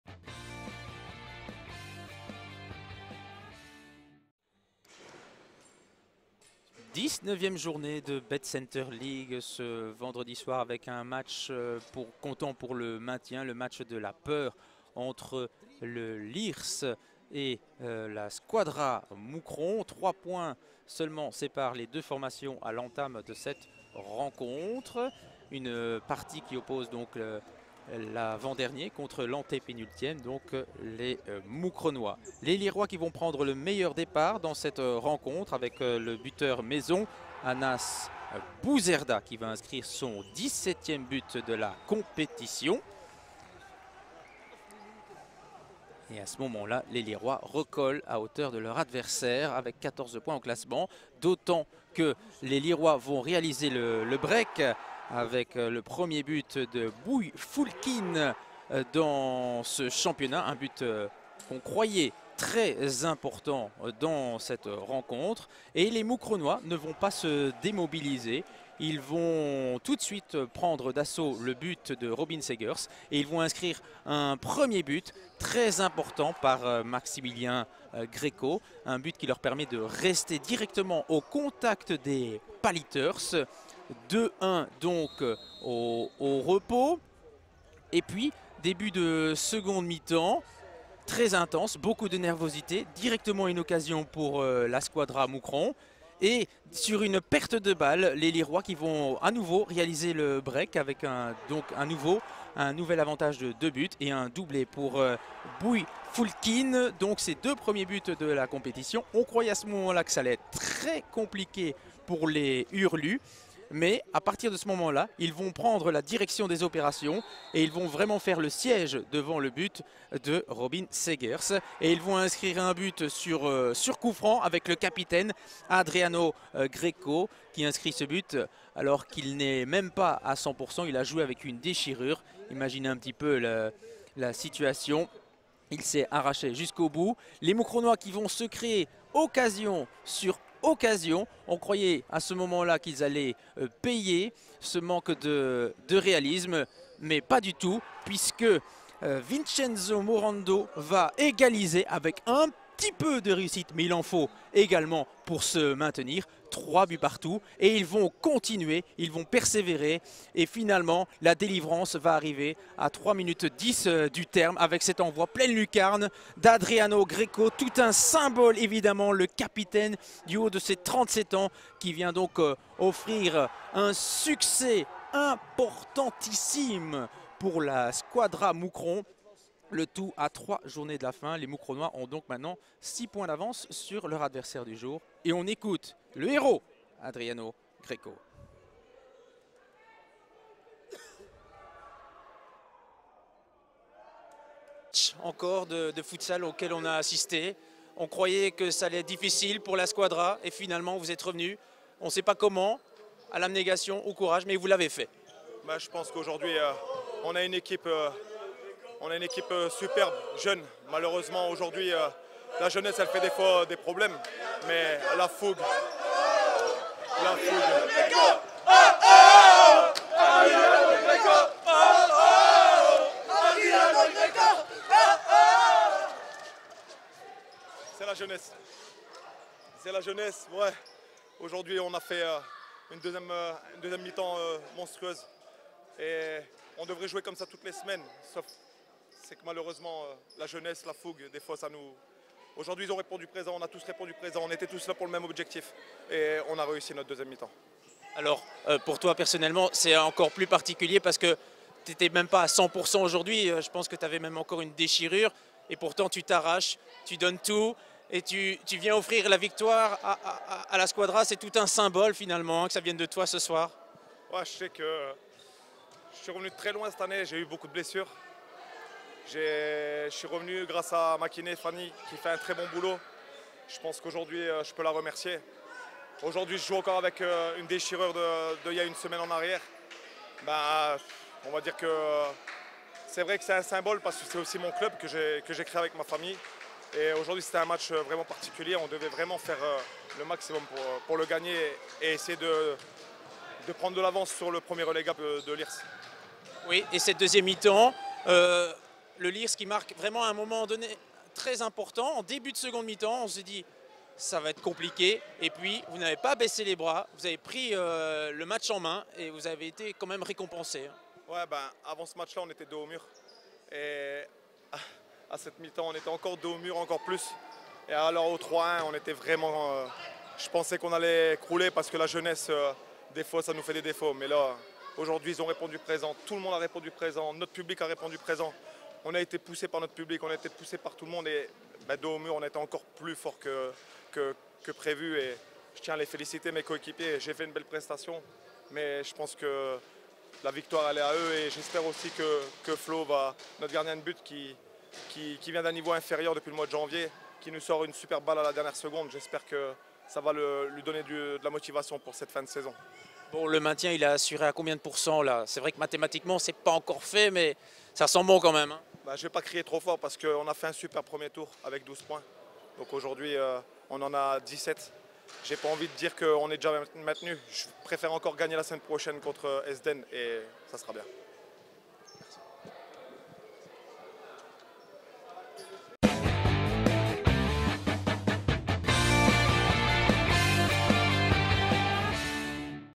Proost Lierse - Squadra Mouscron (FR) | Betcenter Futsal League
11/03/2022 - Découvrez les temps forts de la rencontre entre Lierse et Mouscron.